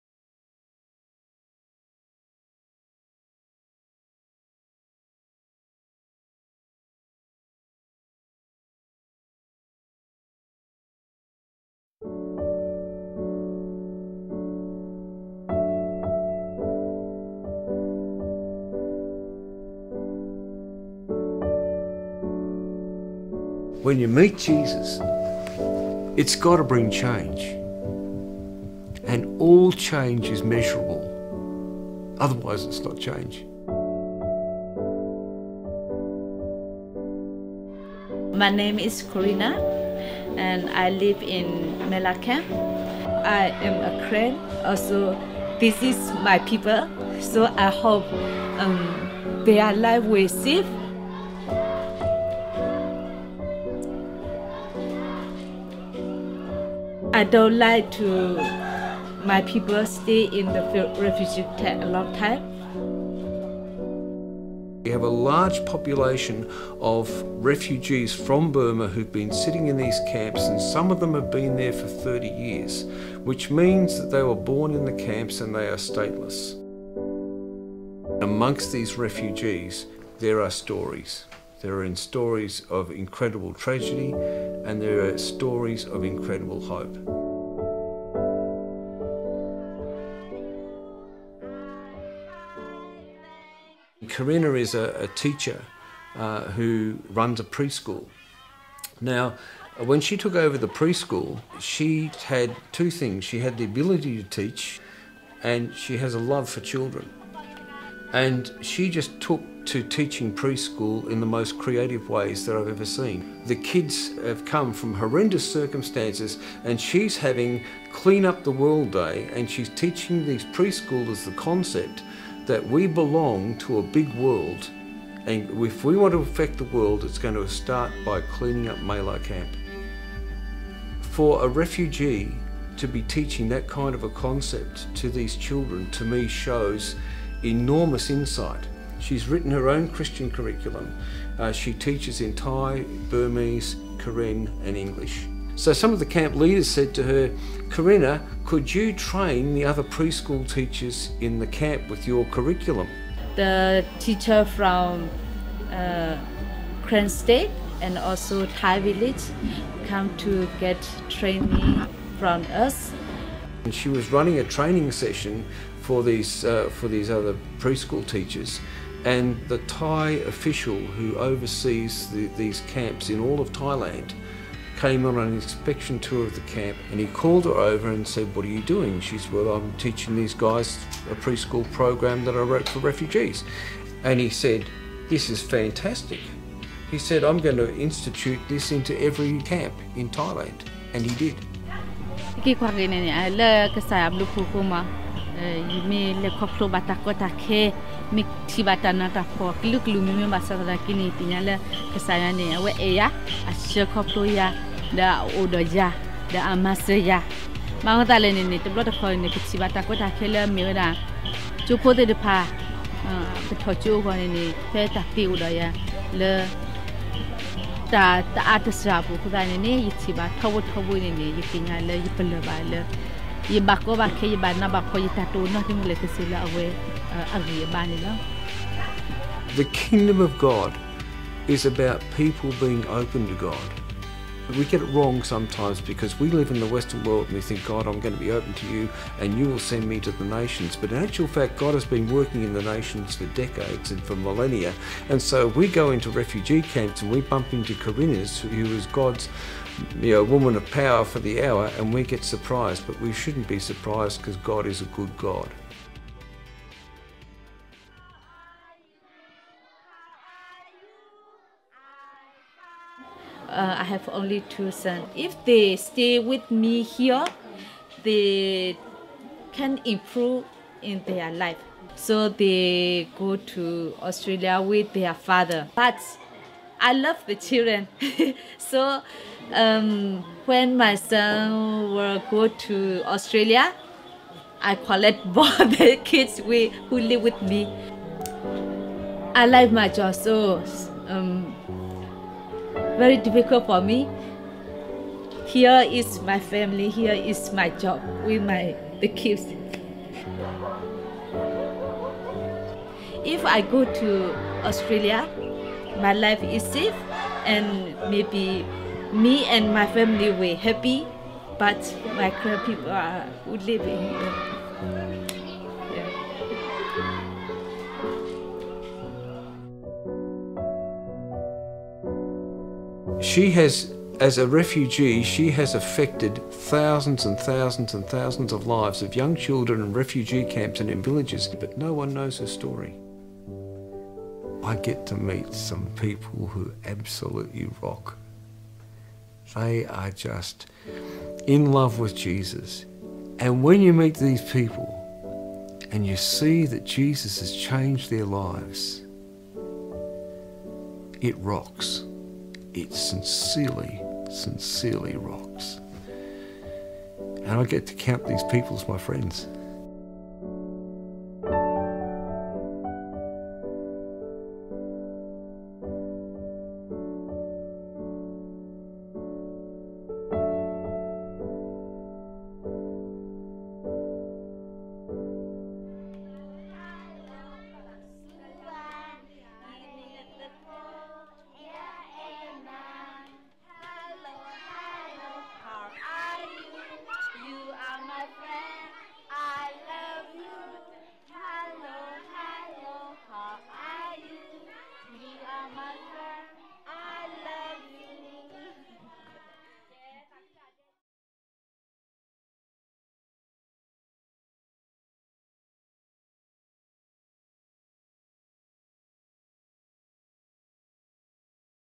Corina's Story HD - Mae-la Preschool
Australian Mercy's work in the preschool in Zone A at Mae-la camp is run by Corina - this is her story. Australian Mercy's National Director, David Skeat, tells Corina's story in this video. (Thanks to the Emerge crew for making this video possible.)

Corina also has a Godson who is not mentioned but will be mentioned when the finl edit is uploaded in about a month.